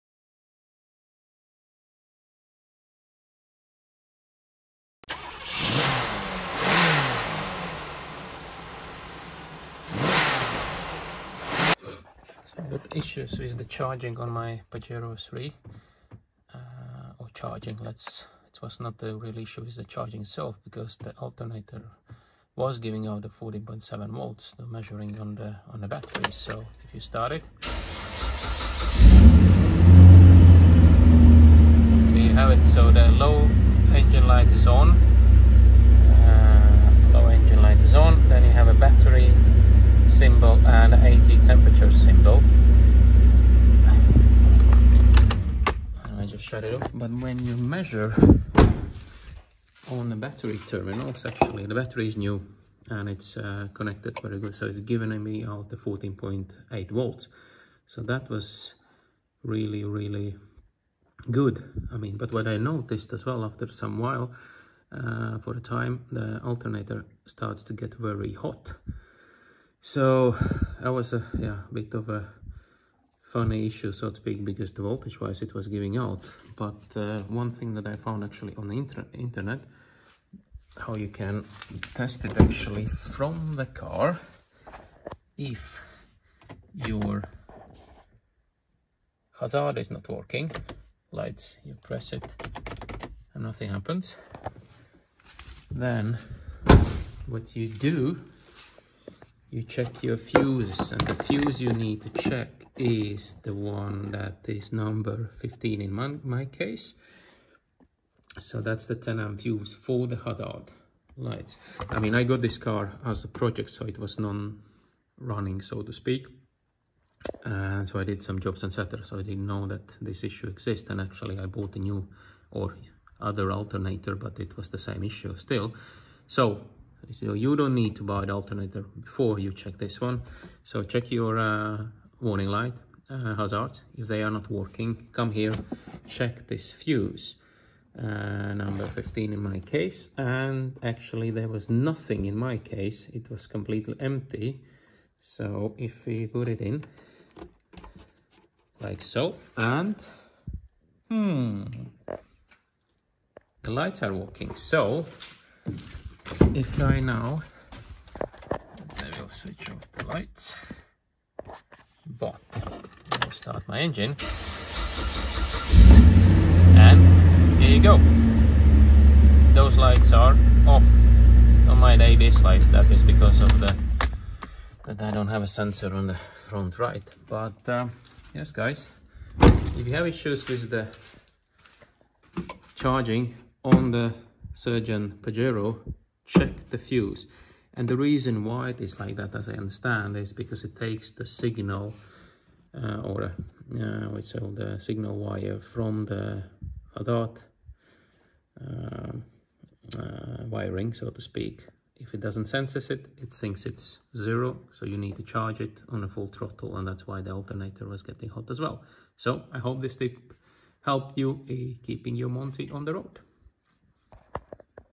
No charge fix for the Mitsubishi Pajero Montero 3
Hi all. Encountered interesting issue on my Pajero 3 when charging light was on, but alternator was charging. Lets see if that can help if you have similar issues.